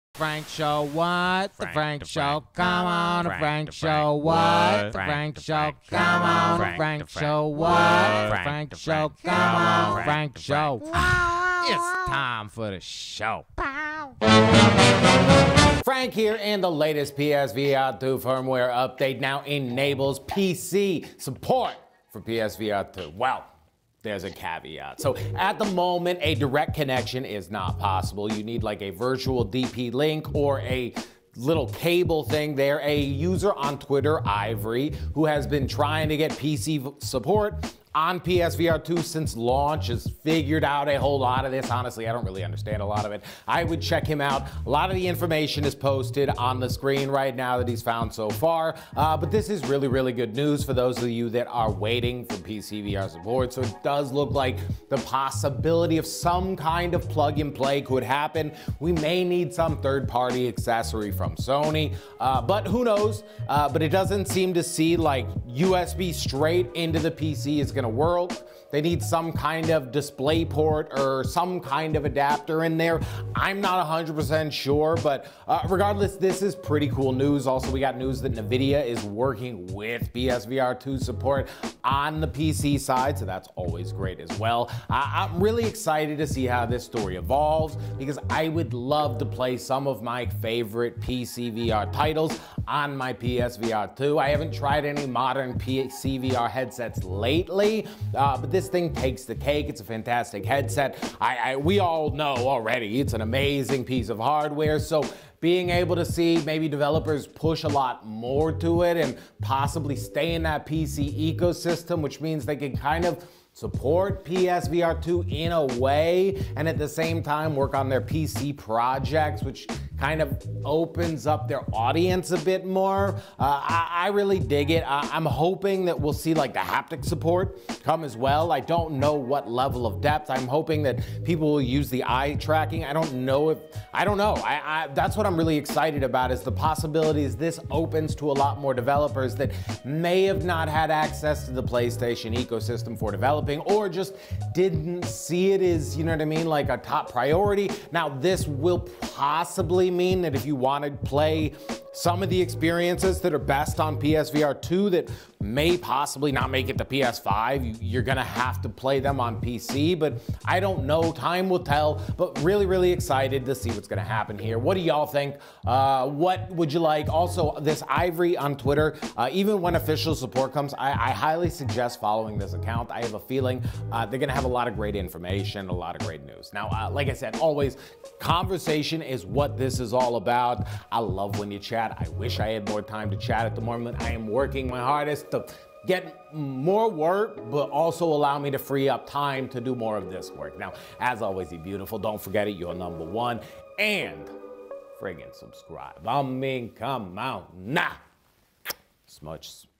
Latest PSVR2 Firmware Update Enables PC Support
The title says it all, the latest PSVR2 firmware update enabled PC support, big thanks for all the info!